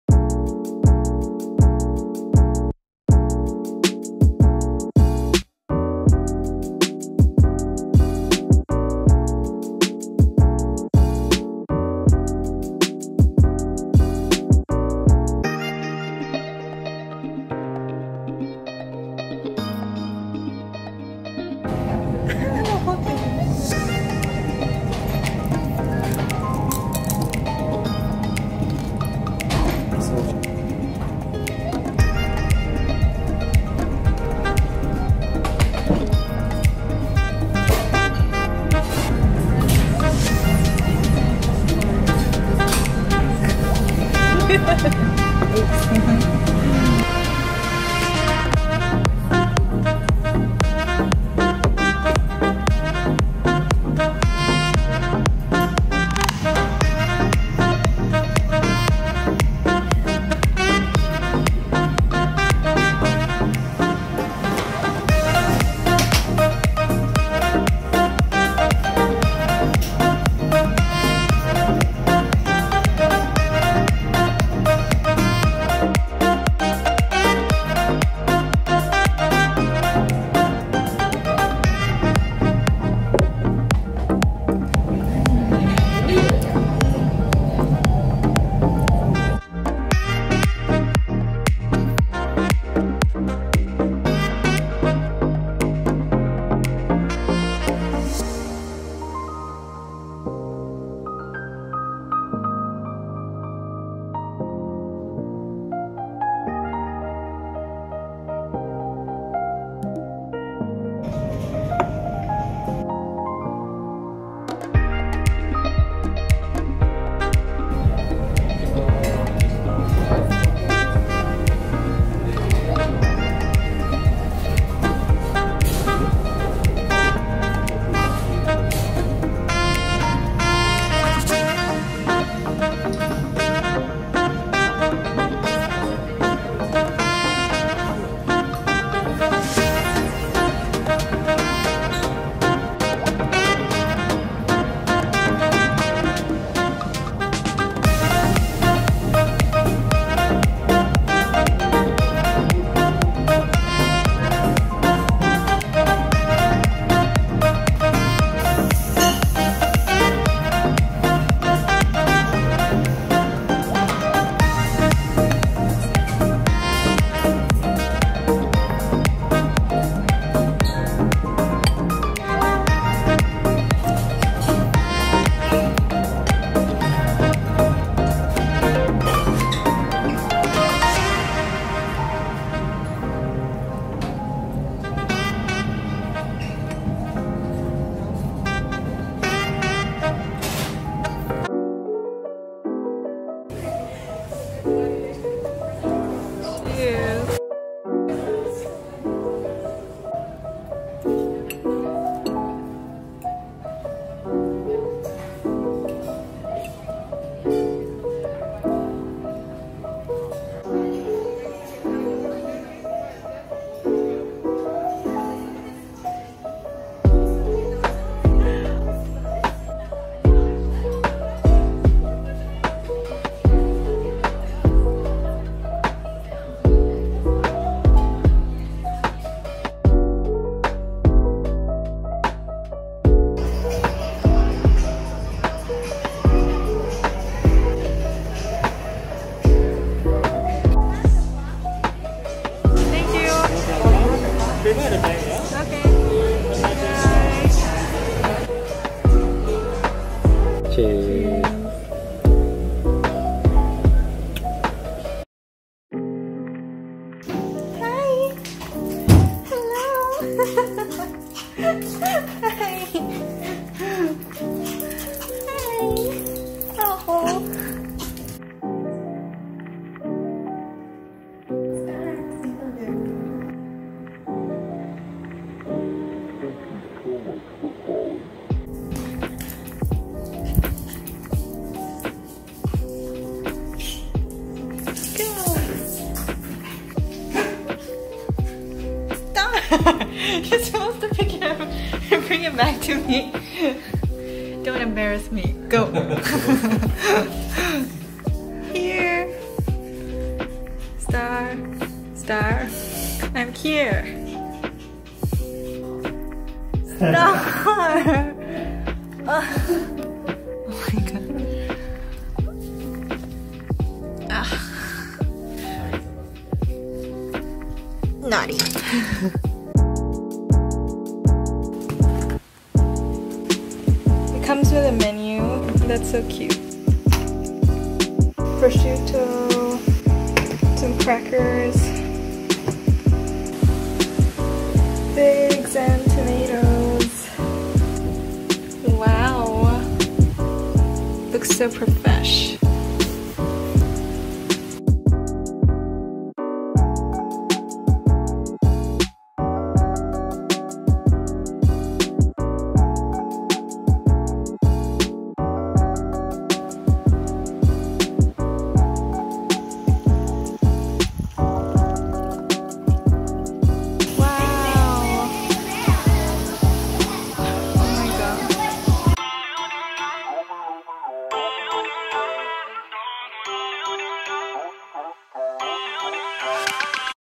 First Weekend Out Post-lockdown 🌈 | Omakase, Date Night, Celebrating Father’s Day 😋❤️
It’s Phase II now in Singapore! I can finally meet my bf!!! And go over to his place to play with his cute doggos. We’re also allowed to eat at restaurants in small groups of max 5 people. I’ve been really craving for both Italian and Japanese food. 🥺 Which cuisine have you guys been craving for the most?

Music🎵